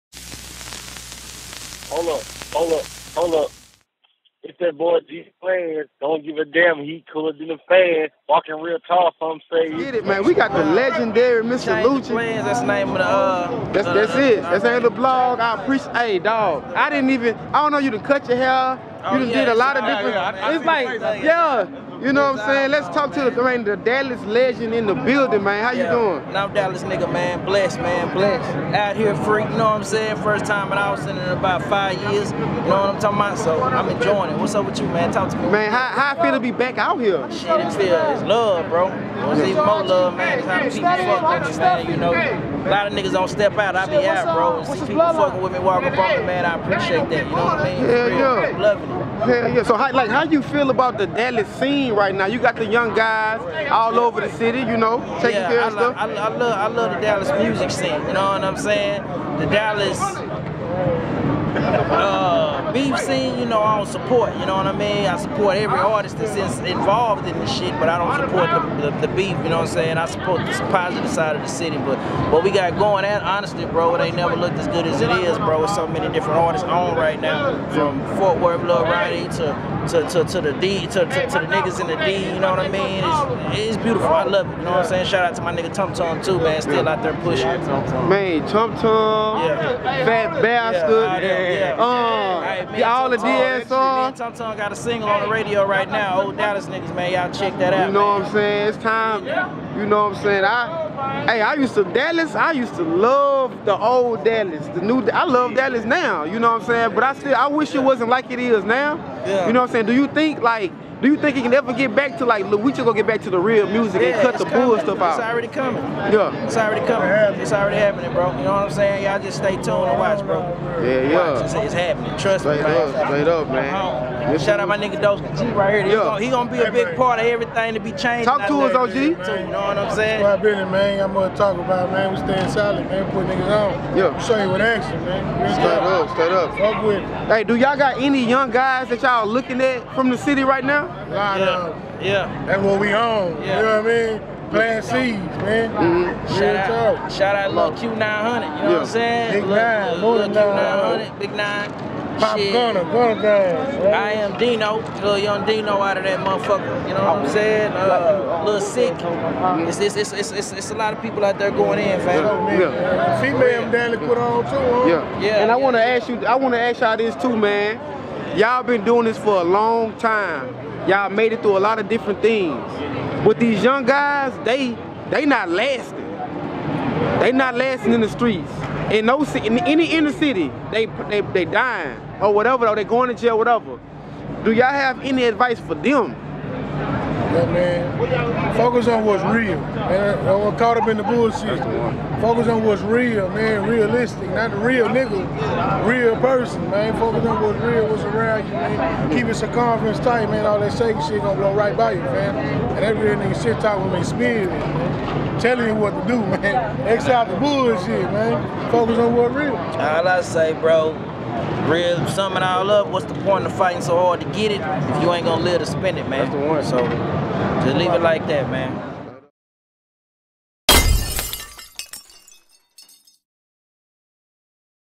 Mr. Lucci " I Do Not Support Dallas BEEF, I Have A Record With Tum Tum." + Dougski G At SXSW 2022.
🌟 Changeofplanz

Dallas Texas legend Mr. Lucci says that he does not support Dallas Beef and more!